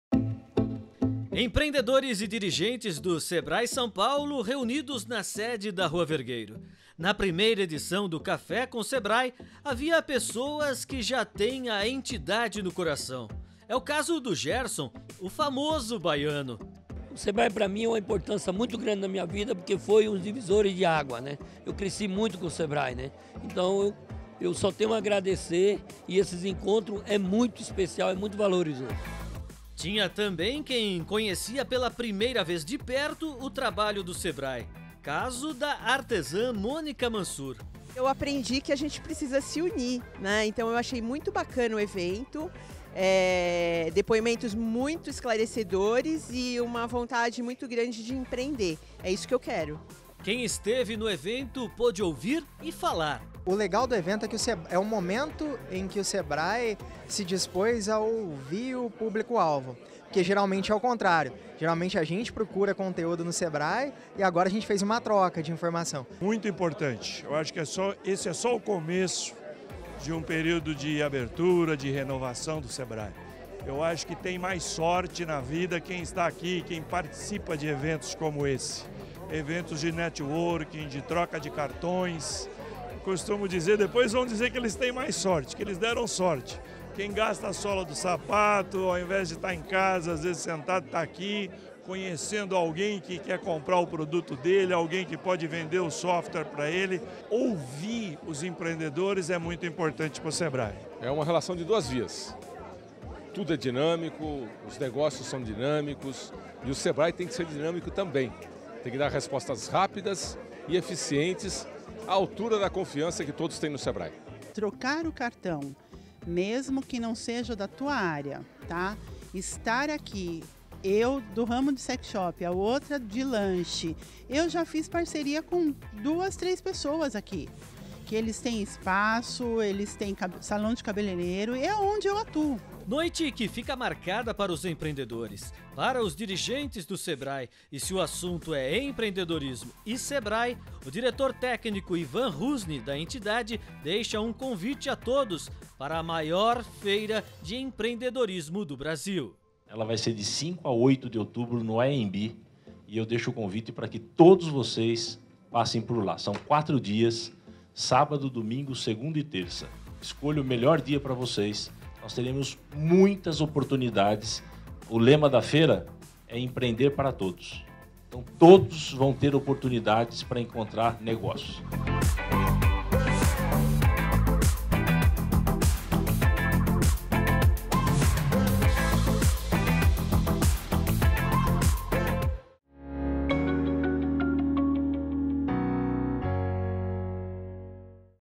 Empreendedores participam da 1º edição do Café com Sebrae
Confira o que aconteceu no primeiro Café com Sebrae, evento que reuniu dirigentes e empreendedores que já são clientes do Sebrae, além de novos empreendedores para uma troca de experiências.